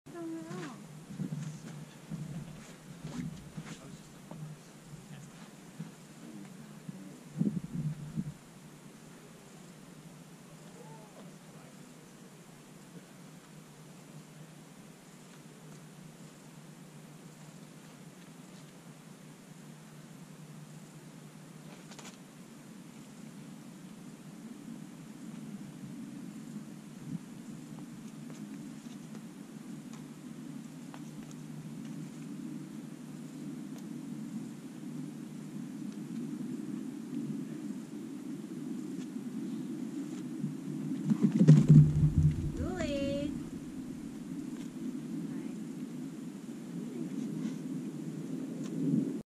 Device Ignitor Right V3 | Moe's Valley
Amazing warmup with the pretty soft V3 in the Teepee area of Moe's Valley, Utah.